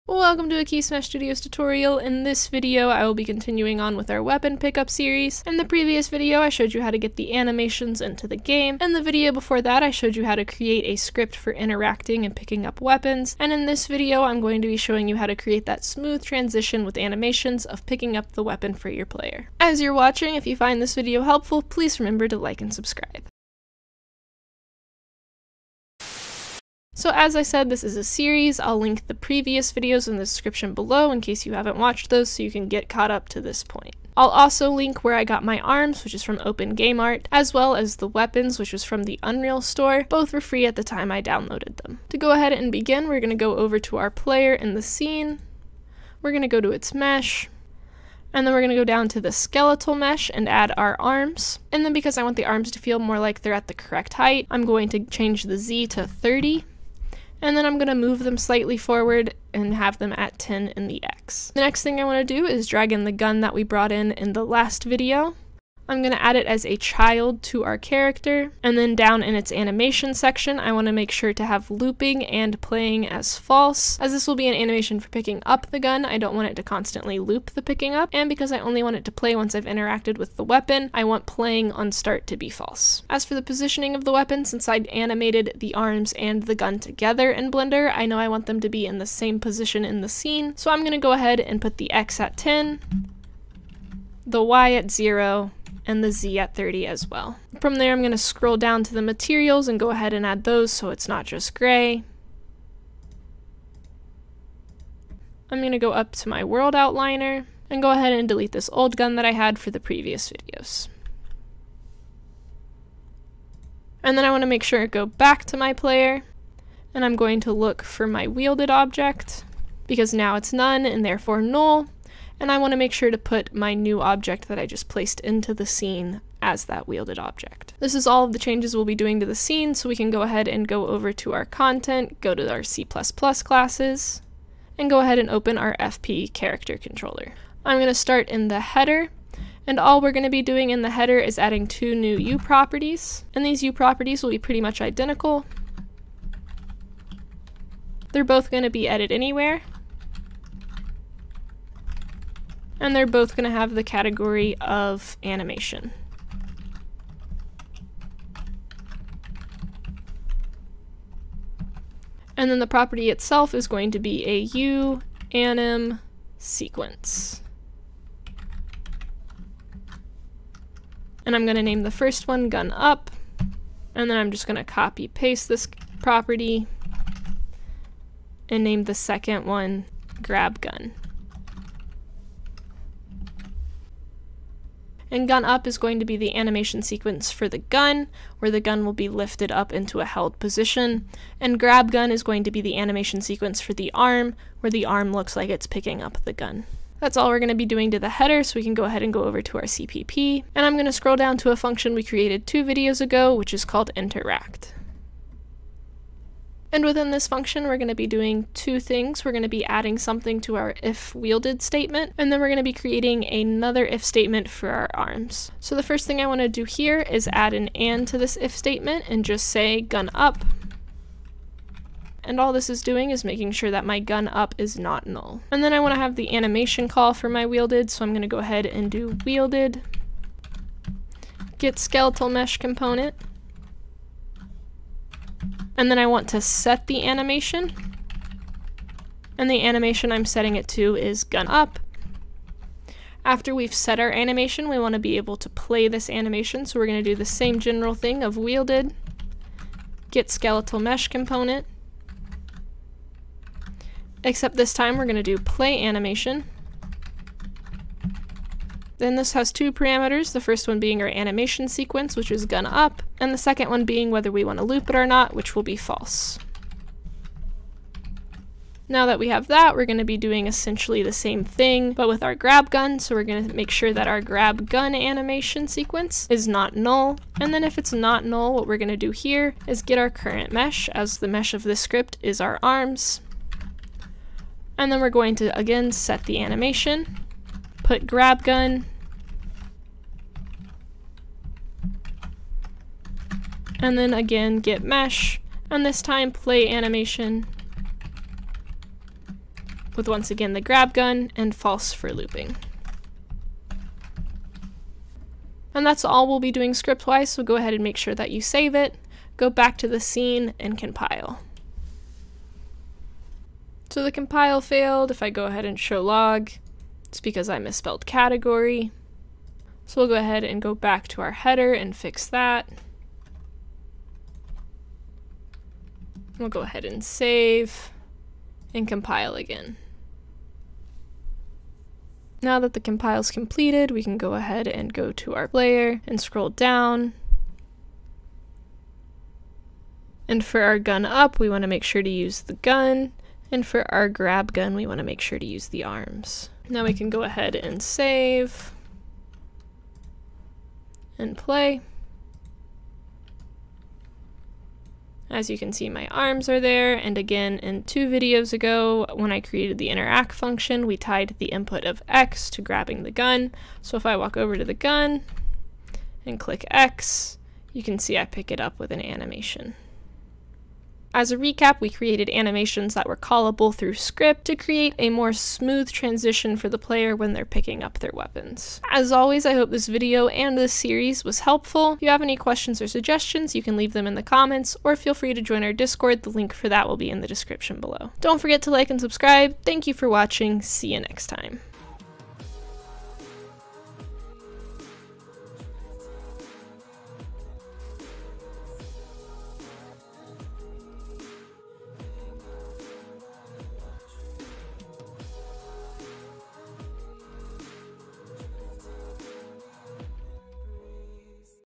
Animating First Person Weapon Pickup in C++ | Unreal Tutorial
Learn how to use animations that are callable in C++.  This tutorial covers how to use Unreal's UAnimSequence property in order to smoothly pick up and wield a weapon through animations.  Just follow along as Emily takes us step by step through an easy process to activate an animation for wielding a weapon.